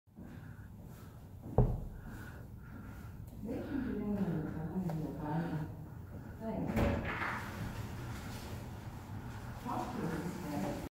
Orangutan clapping
8 year old orangutan clapping